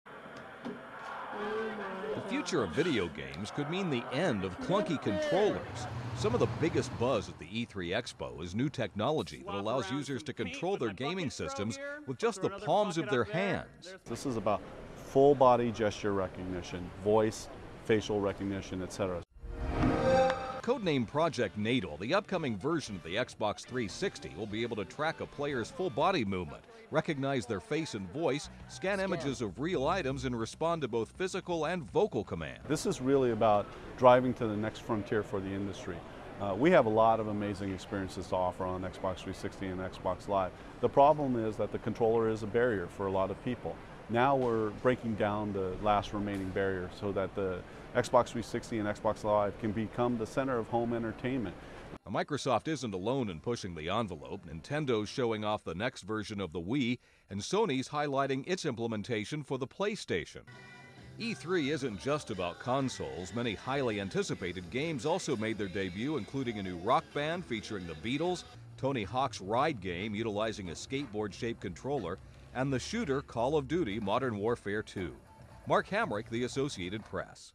E3 Report: Where's My Controller?
Microsoft kicked off the Electronic Entertainment Expo with a bang, displaying a new technology that allows users to control their Xbox 360s -- without any handheld controllers. (June 3)